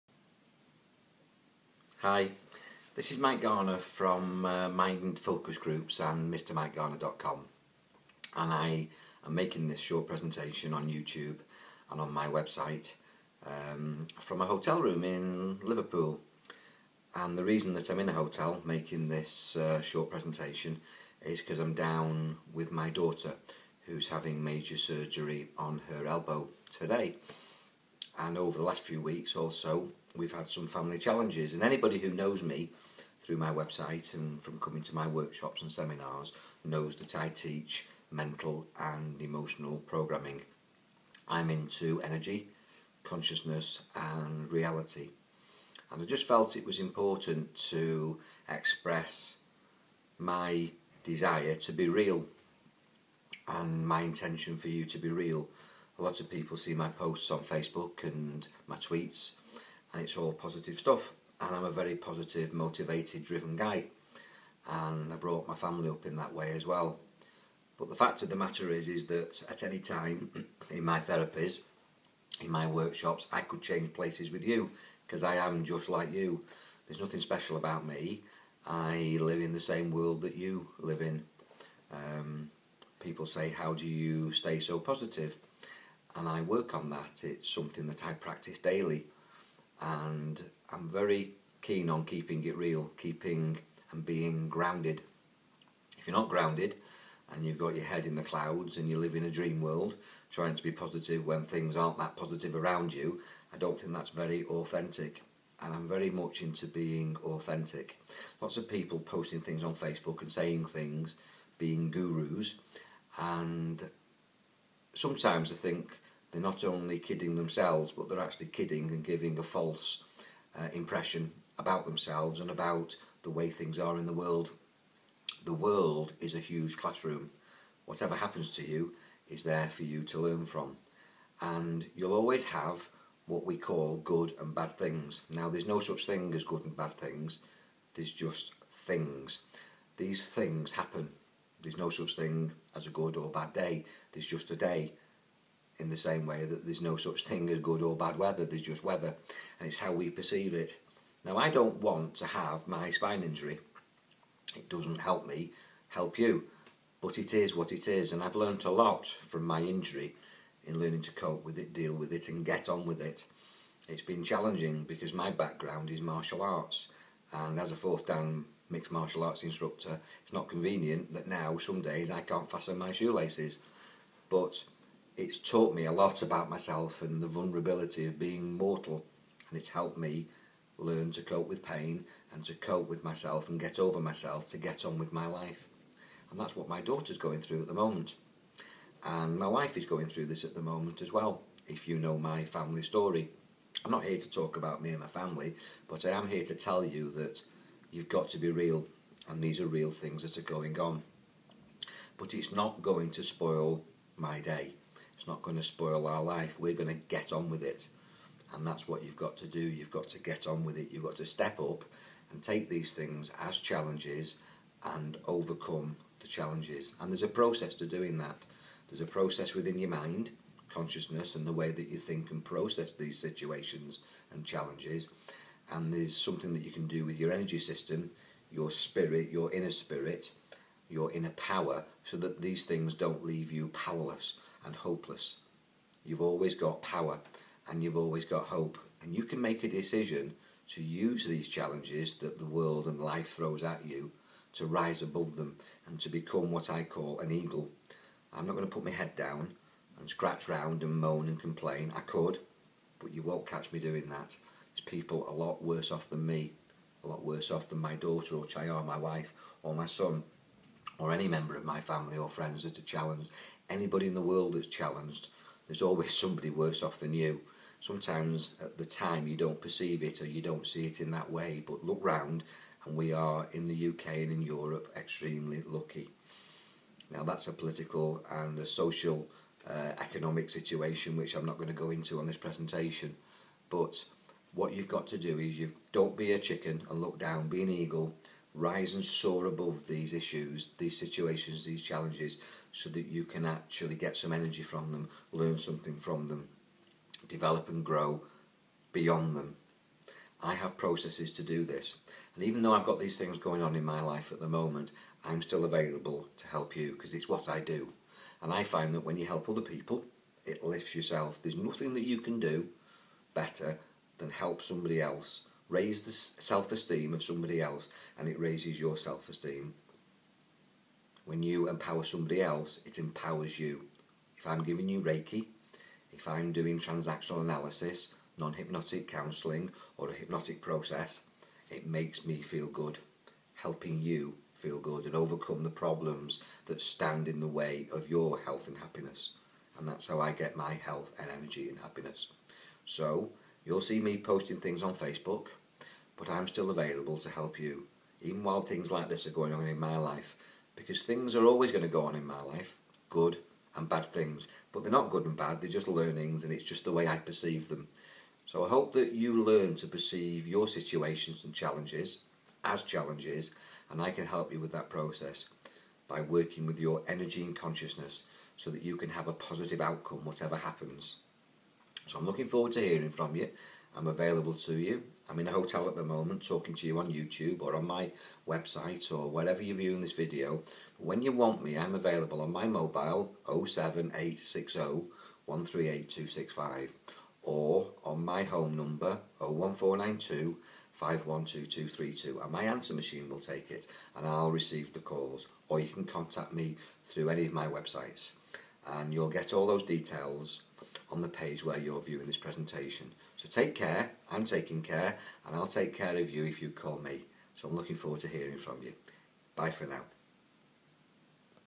Challenges are Opportunities - Mr Mike Garner
Made on 18th March 2013 when my daughter was having surgery - Watch this - It's still massively valid:
Being Real: Life at times can be challenging but the truth is that there is no such thing as good or bad situations, it's just  matter of how you process them.